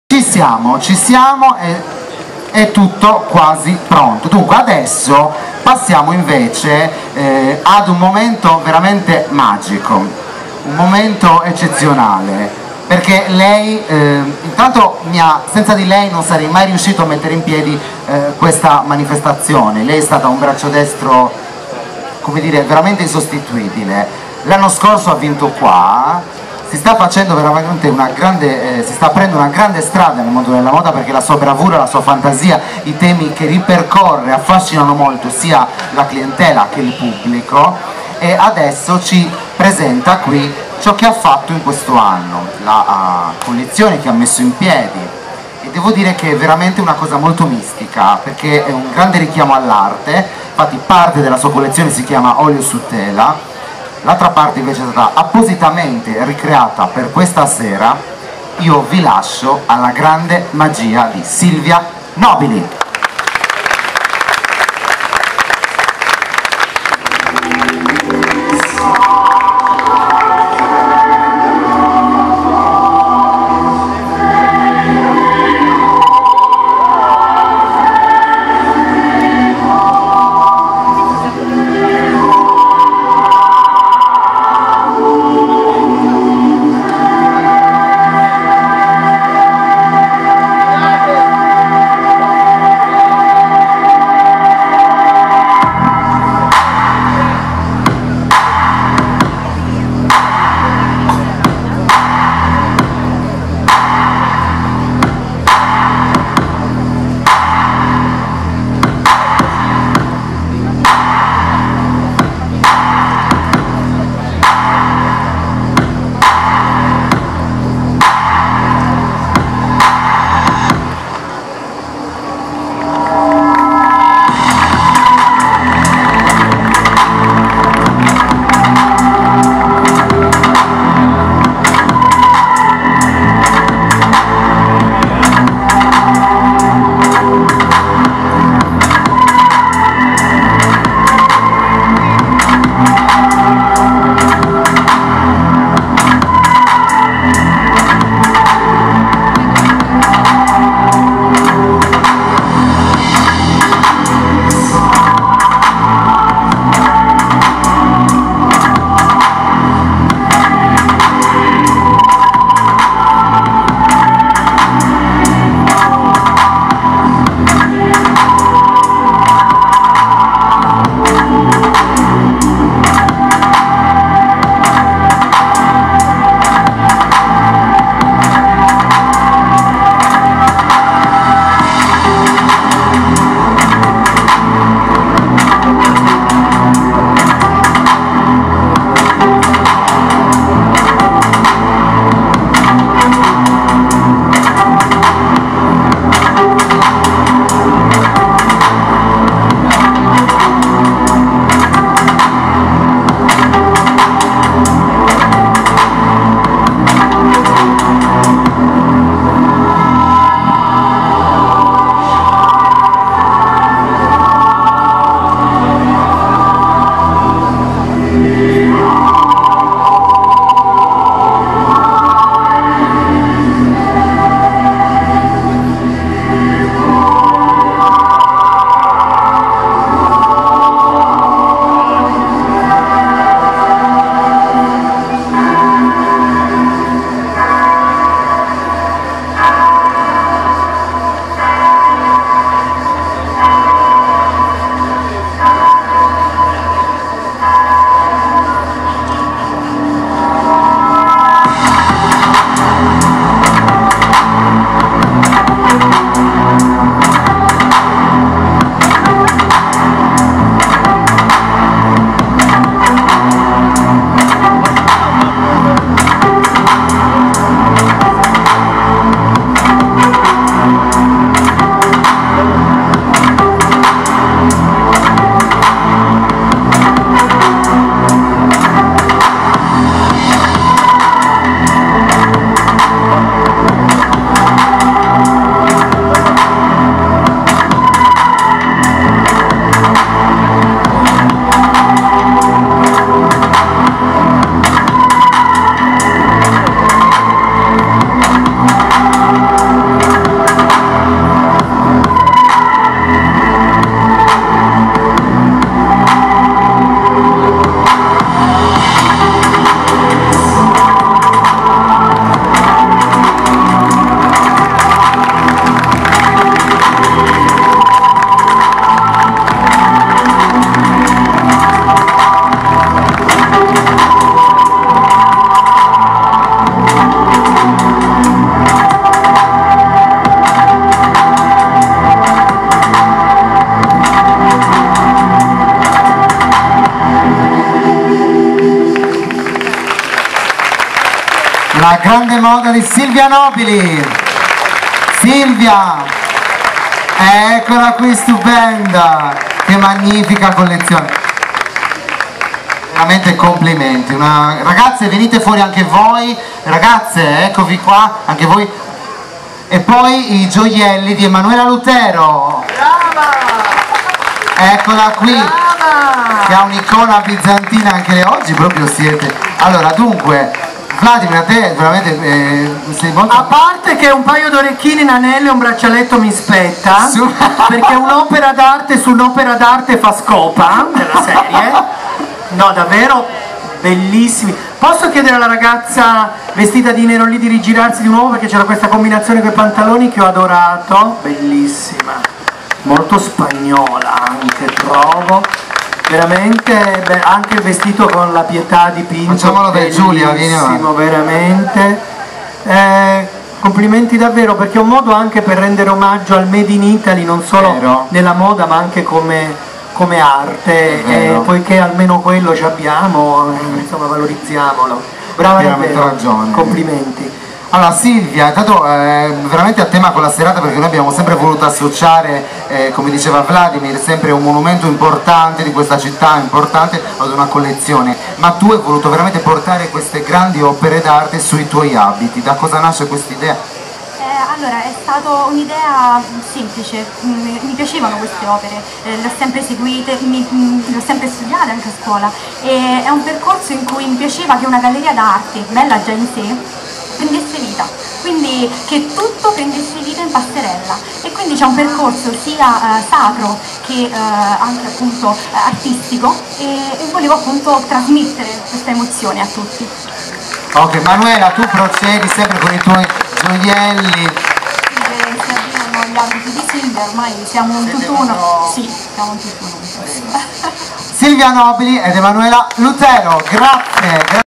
Finale Oltemoda 2015 - Ospiti speciali Silvia Nobili ed Emanuela Lutero
Ad un anno dalla vittoria del concorso Oltremoda by U/nite la stilista Silvia Nobili mostra al pubblico del Gay Village la sua nuova collezione accompagnata dai gioielli di Emanuela Lutero.